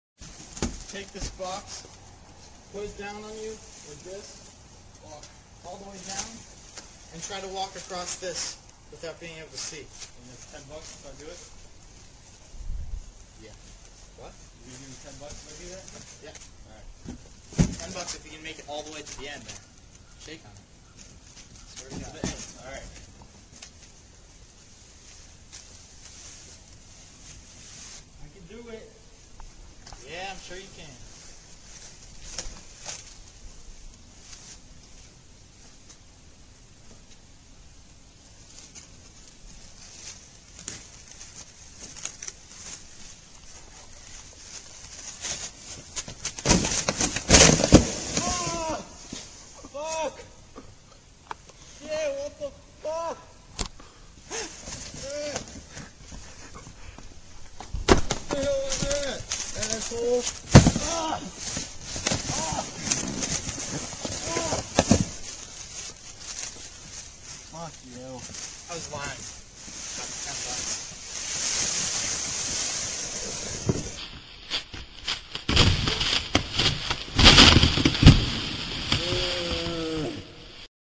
Box Challenge - 0wned!
Kid takes up the challenge for 10 bucks!

idiot lol